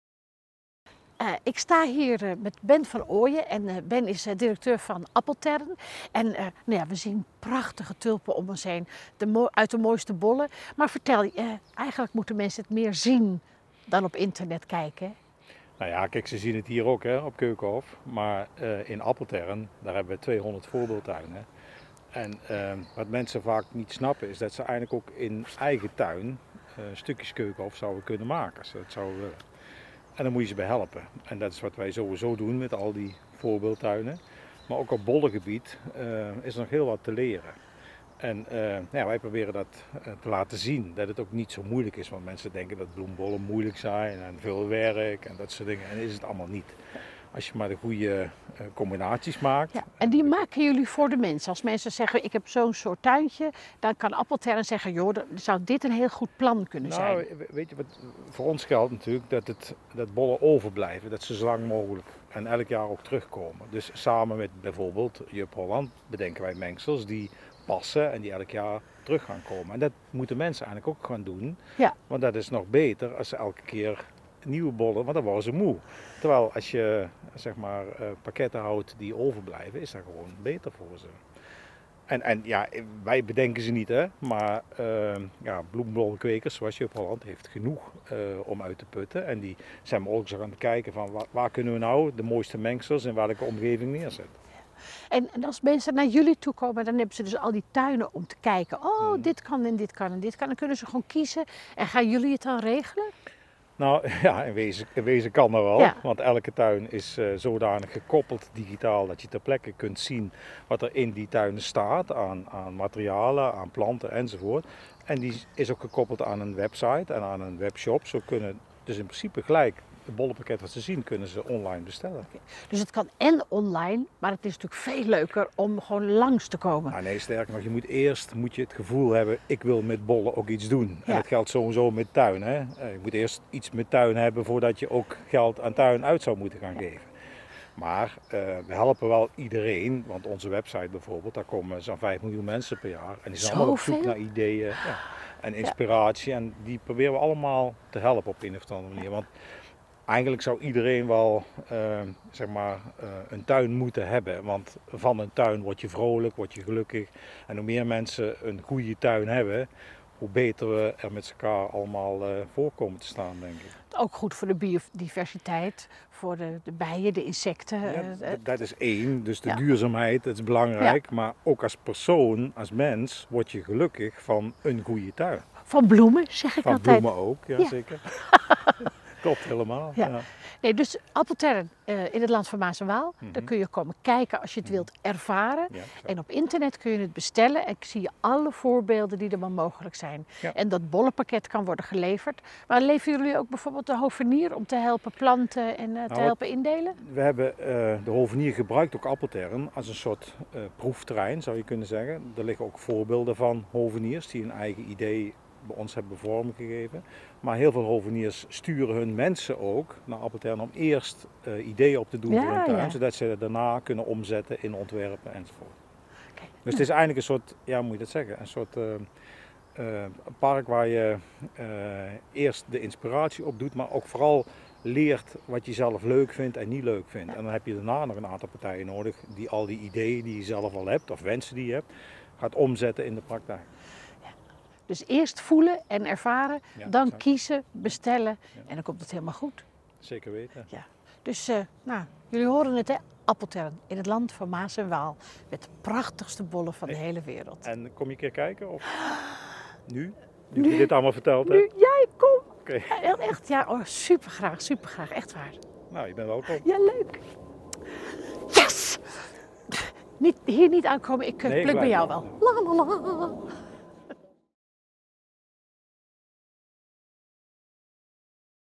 Karins Tuintips - Appeltern
Toen ik onlangs een gezellige middag in de Keukenhof doorbracht, sprak ik ook met Ben van Ooijen, de directeur van de Tuinen van Appeltern. Ik vond het zo enig om te horen hoe hij met zijn passie voor tuinieren een inspiratie is geworden voor menig tuinliefhebber. Het is toch te leuk dat je door de vele voorbeeldtuinen naar hartelust inspiratie kunt opdoen voor je eigen tuin? 🥰💋💐